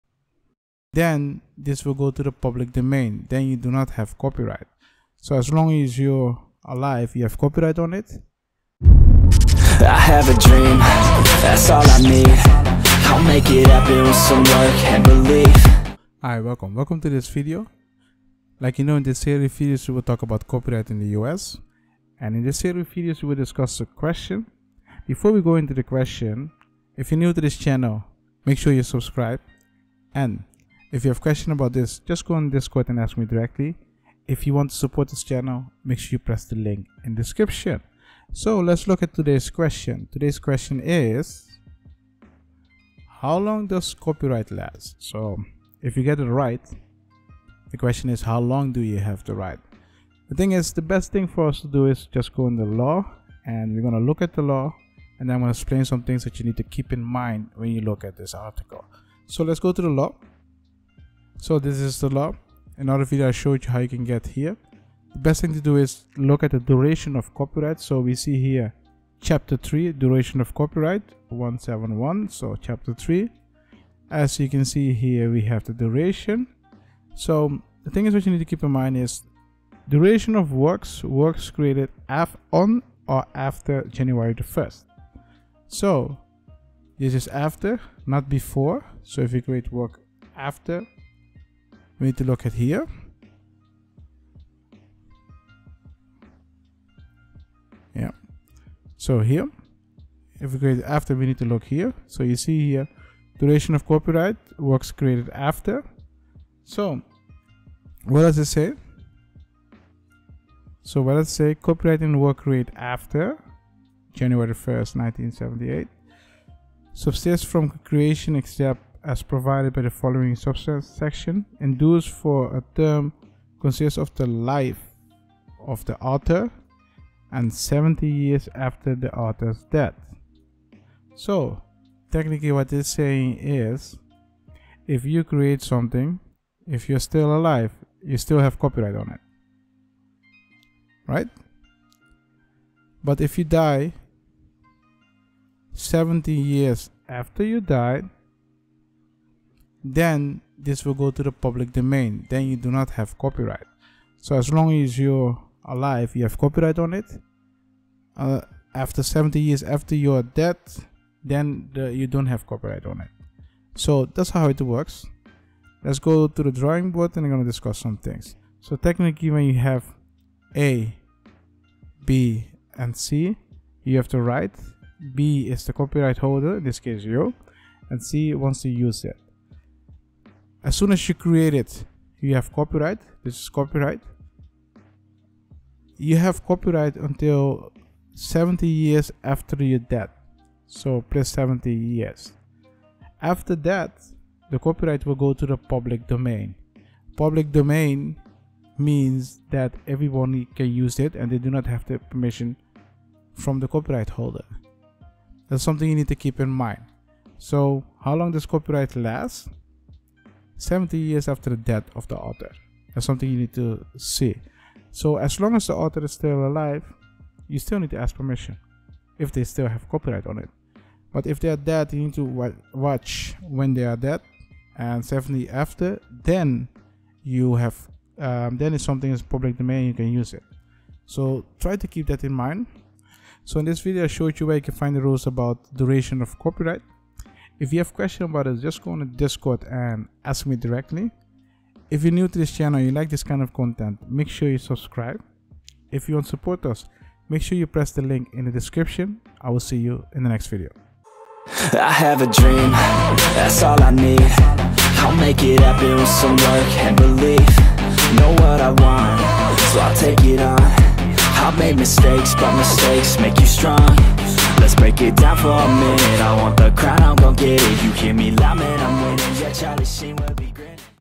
How Long Does A Copyright Last? | Martisz [ Copyright USA ]
In today's video, we're discussing Copyright USA. This video will be very helpful for people that want to learn more about Copyright in the USA.